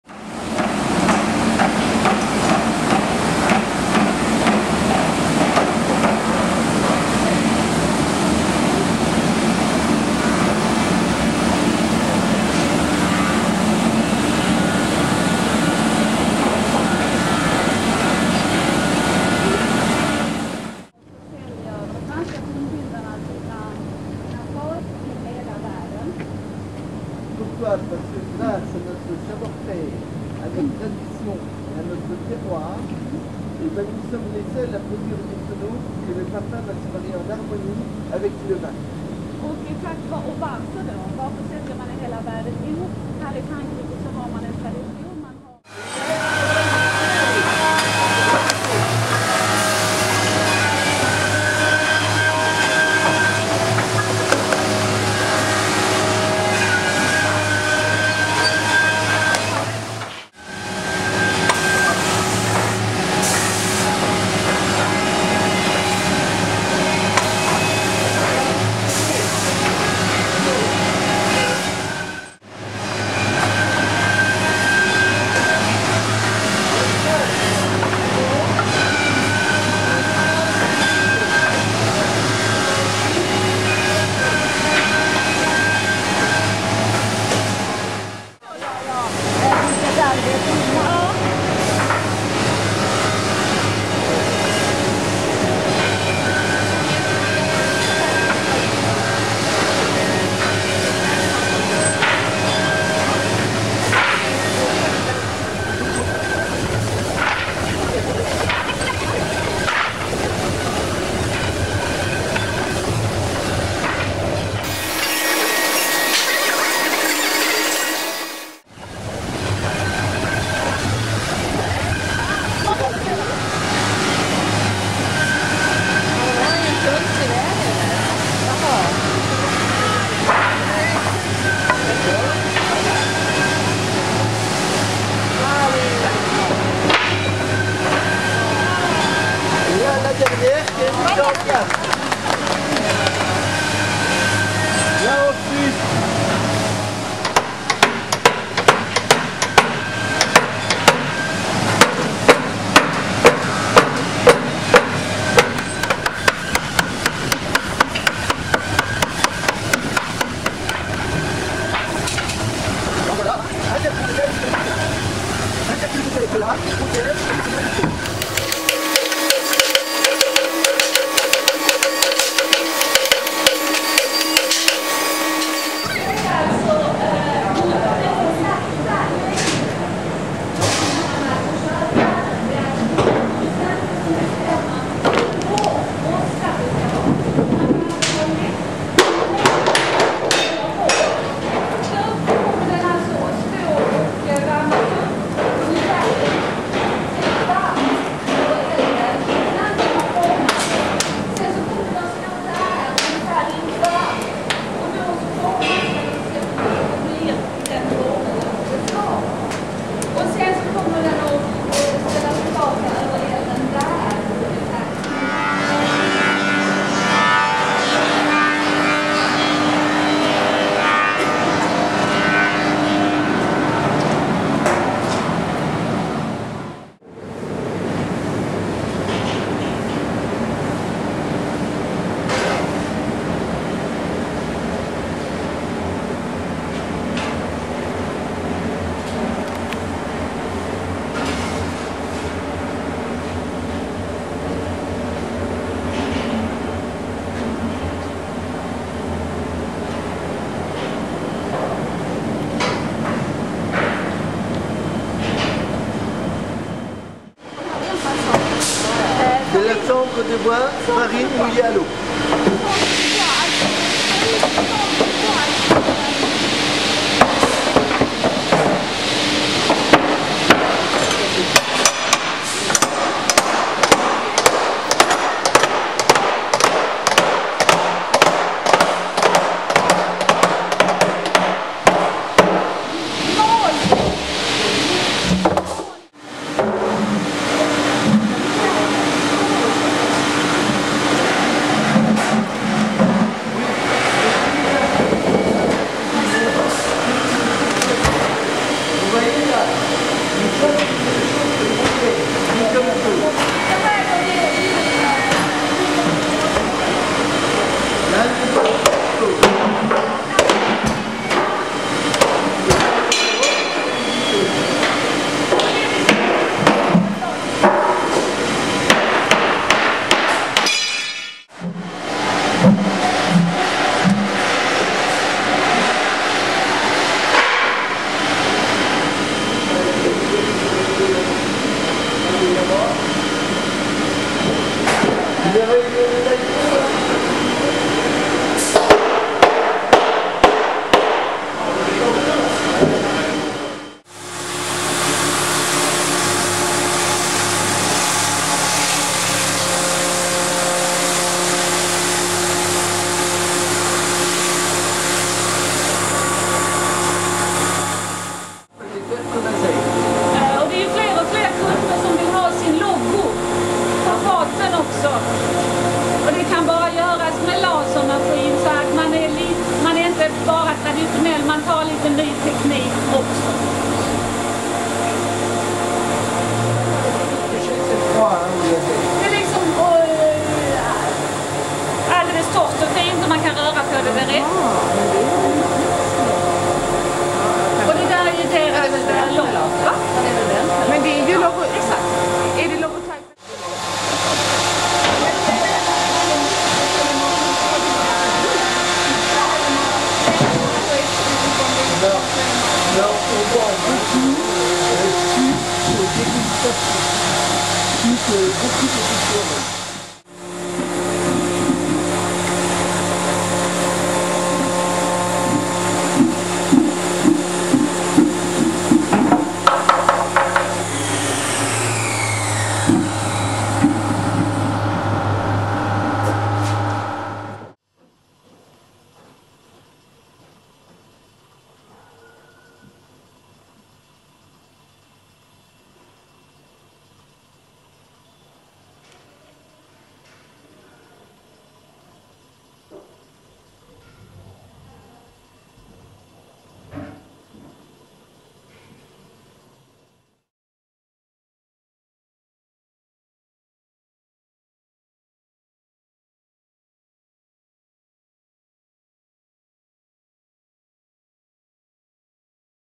Making a wine barrel at C Gillet Cooperage
*Come on a Wine & Food Tour with us!* --- The making of a wine barrel at C Gillet Cooperage.

Tonnellerie Claude Gillet (cooperage) shows us how a cooper creates a barrel. An oak wine barrel (barrique, pièce) is made: from oak planks; cutting them in shape, assembling the staves (planks) to a barrel, putting on the metal hoops that keep the barrique (if it's the "Bordeaux size" of 225 litres. Pièce if it's the  together Burgundian 228 litre barrel) together, toasting it, i.e. burning the interior, called 'chauffe' in French to give it the characteristics required, and putting the final touches to it. Tonnellerie Claude Gillet is based in Saint Romain in the heart of Burgundy, France.

Hur man tillverkar ett ekfat för fatlagring av vin. Tunnbindaren Claude Gillet visar hur det går till -- från ekplankor till färdigt vinfat.

Tonnellier Claude Gillet à Saint Romain en Bourgogne nous montre comment une barrique bordelaise, ou une pièce bourguignonne, est construit.